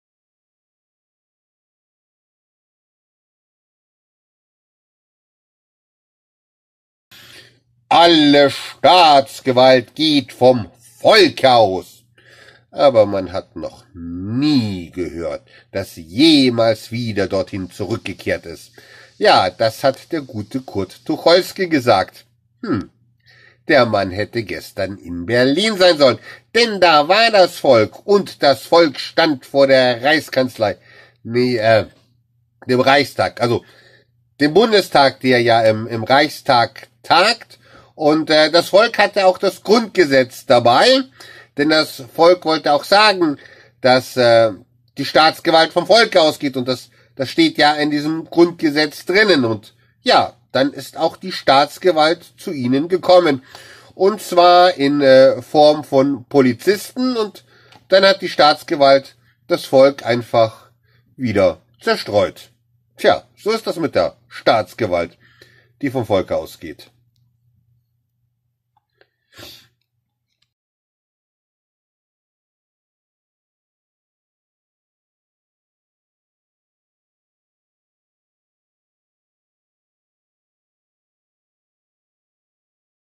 Alle Staatsgewalt geht vom Volke aus - Aber Wehe wenn sie zurückkommt !!!
„Alle Staatsgewalt geht vom Volke aus, Doch man hat noch nie gehört, dass sie jemals dahin zurückgekehrt ist.“ hat Kurt Tucholsjy gesagt. Da hätte er mal gestern in Berlin sein sollen. Da war das Volk, und das Volk stand vor der Reichskanzlei, oder wars der Reichstag. Ja und sie hatten das Grundgesetz dabei. Also das Büchlein, in dem Steht, dass alle Staatsgewalt vom Volke ausgeht. Und dass ist die Staatsgewalt in Form von Polizisten gekommen und hat das Volk zerstreut.